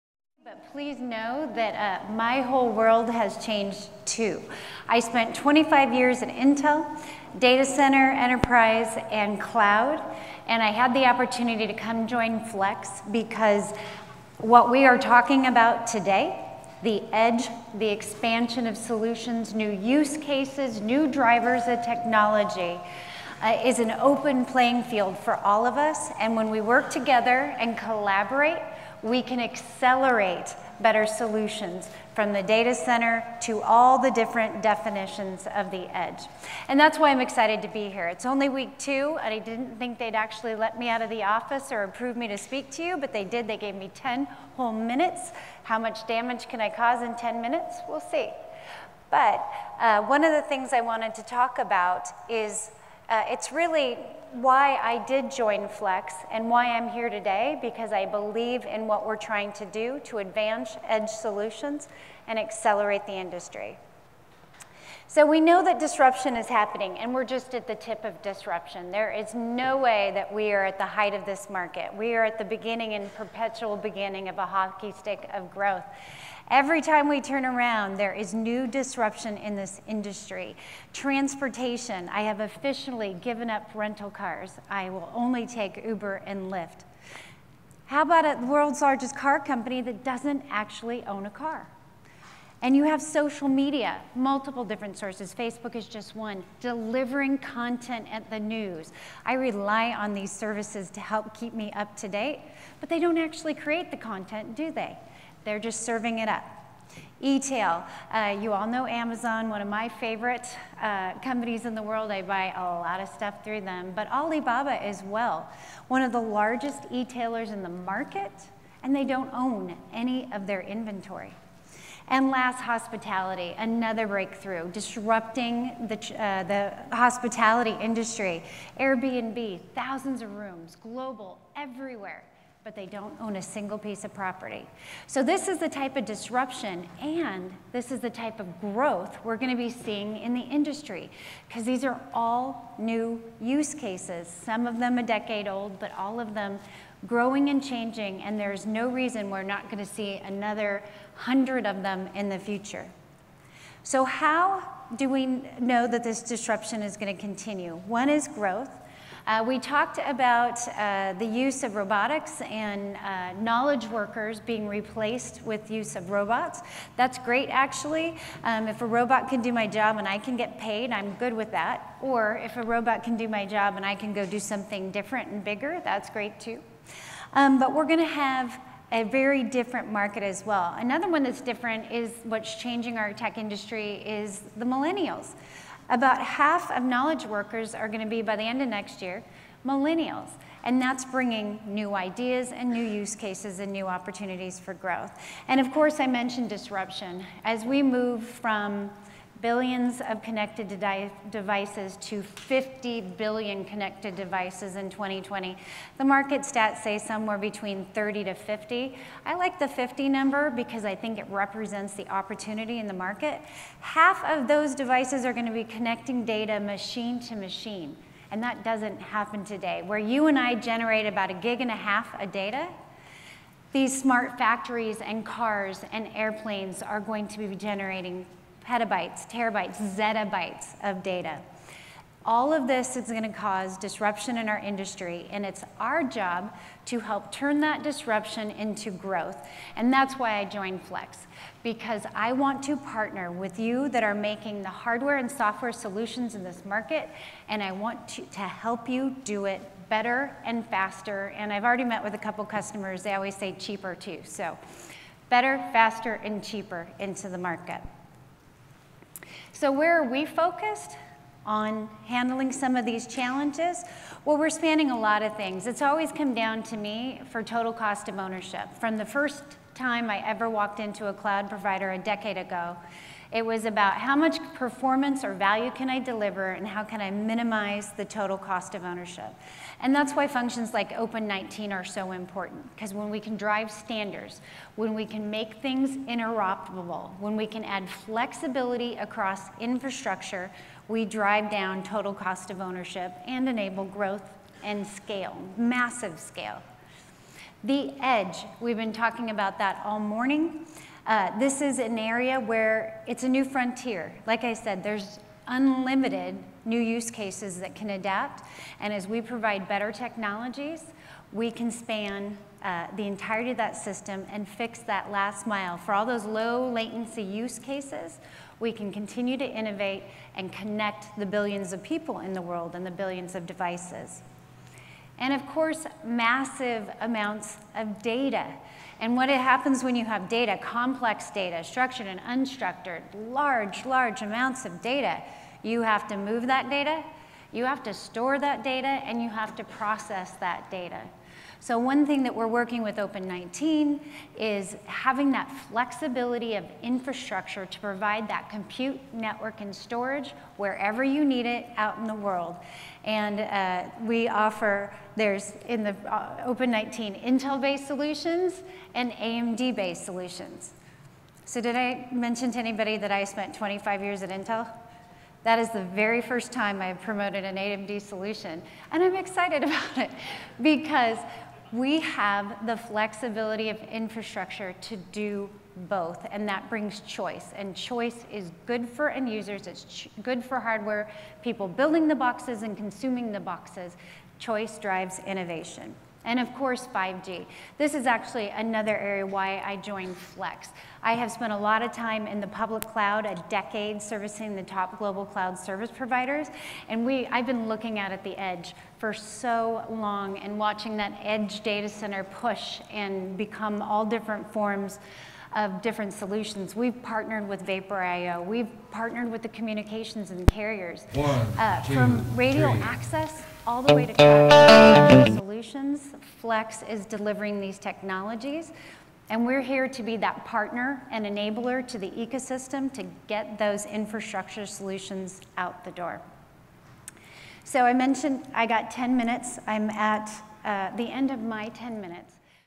Summit 2019 - Cloud & Data Center in 2020
Raejeanne Skillern, President of Communications & Enterprise Compute, Flex